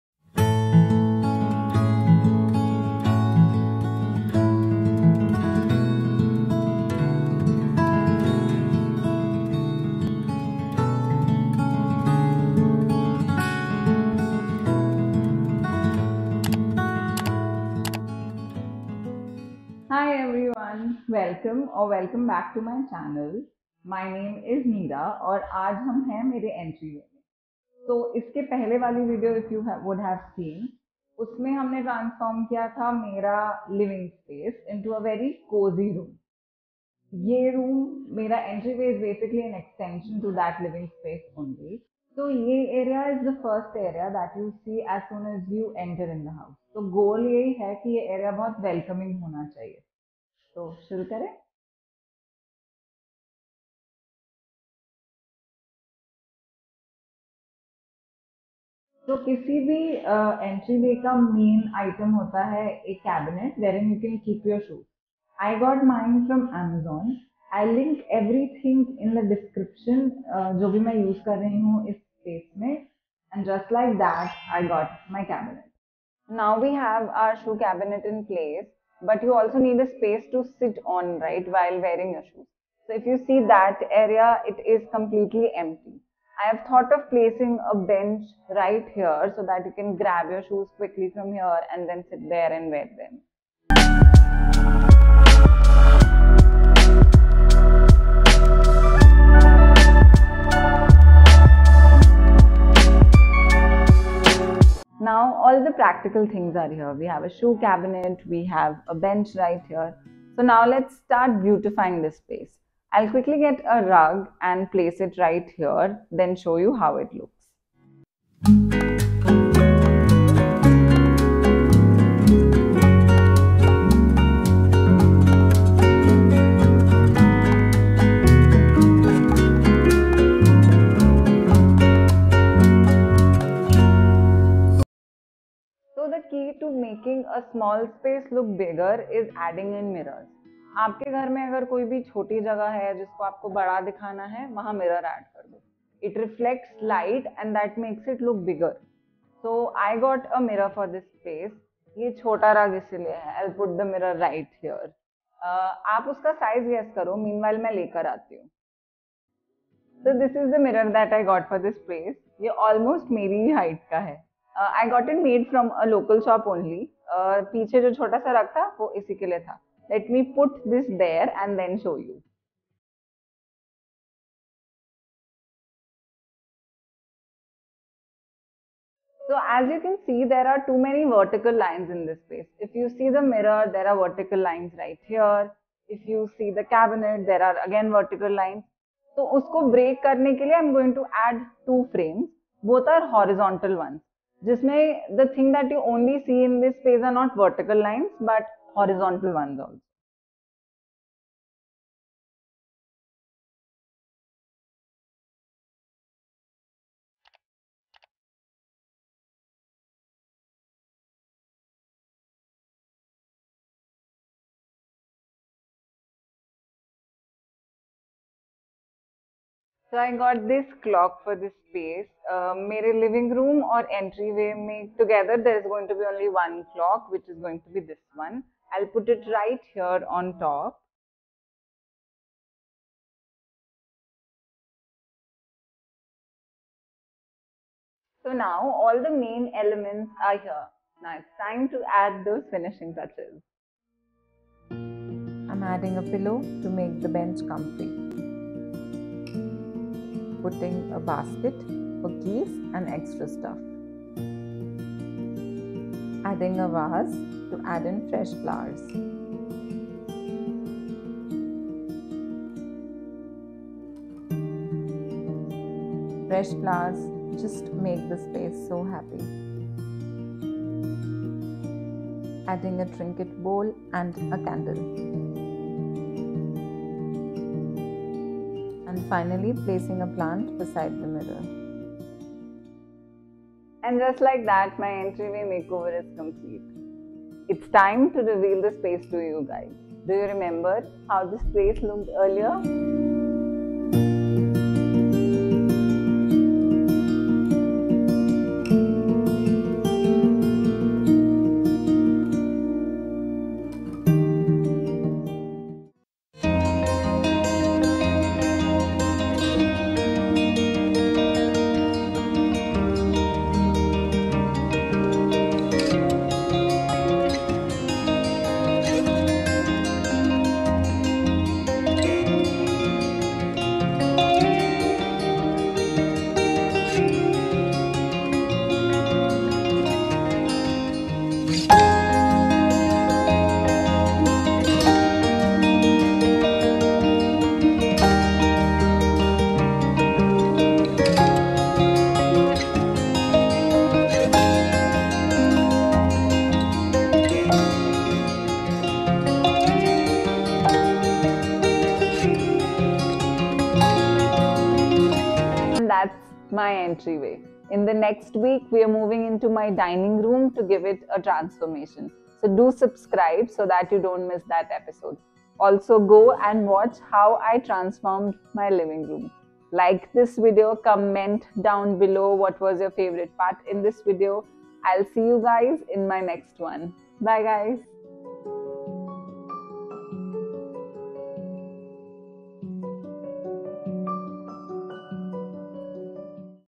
Entryway Makeover | Foyer Makeover | * 6 Feet Tall Mirror & More * | Neera Mishra
I Loved creating this welcoming space that I call my entryway now.

 ~ ♡ Neera

Shop My Entryway ♡
• Shoe Cabinet:
1. Style 1
2. Style 2
• Wall Clock
• Trinket Bowl : Ikea (AROMATISK)
• Key Basket : Westside